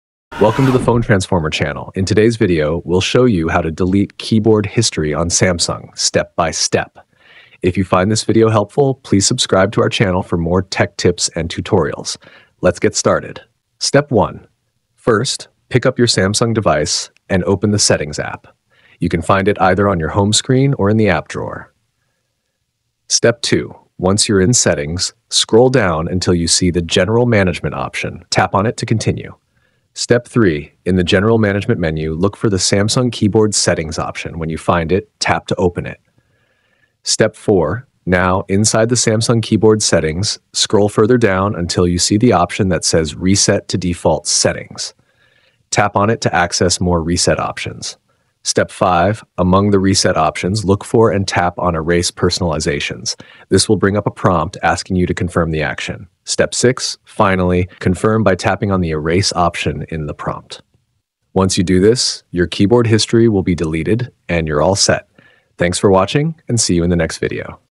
How to Delete Keyboard History in Samsung
Samsung smartphones store a history of words, phrases, and suggestions from your keyboard to improve typing accuracy and speed. While this can be helpful, it may also become a privacy concern if you want to clear your typing history. Deleting the keyboard history can help you maintain privacy and clear outdated suggestions. In this video, we guide you through the process of deleting your keyboard history on a Samsung device, ensuring your device stays clutter-free and secure.

- Delete Samsung keyboard history
- Clear keyboard suggestions Samsung
- Erase typing history Samsung
- Remove keyboard history Samsung
- Samsung delete keyboard cache